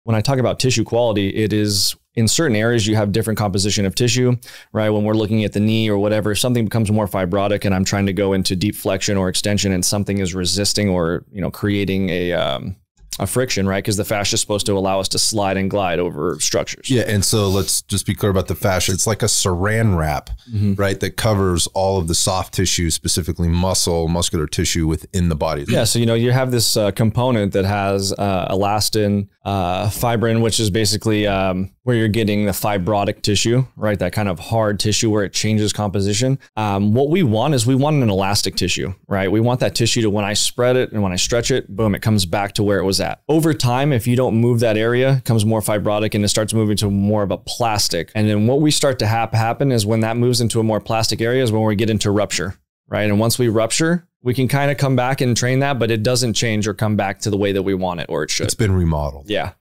Tissue Quality
Welcome back to Iron Sights After Dark. We’ve heard from performance coach & trainer Jeff Chenault before but in this episode, I brought him in for his first After Dark appearance to discuss performance training for the tactical athlete. Chenault discusses his background and how his search to alleviate pain sent him on a path to study the human body, root causes of pain & a deep understanding on how the body works. Jeff discussed fascia, holistic systems of the body & relates his knowledge to performance training for the functional tactical athlete who wants to achieve maximal performance. For a long time pushing the limits was the primary training strategy for active service members - research & performance training is creating a different breed of tactical athletes. Over the years, I’ve helped countless first responders increase their performance on the job. In this episode, both Jeff and I combine our experience as experts in human performance to share our best theories for how the tactical athlete can use proven concepts in the field of health & performance to increase their athletic performance and longevity both on and off the job.

Topics Discussed:

Jeff’s Background
Pain & Addiction
ELDOA & Tissue Quality
Fascia & Tendons
Equipment & Resources
Joint Stability & The Pelvis
Ego & Lifting
Passive vs. Active Range of Motion
Shooting Implementation
More Iron Sights Interviews: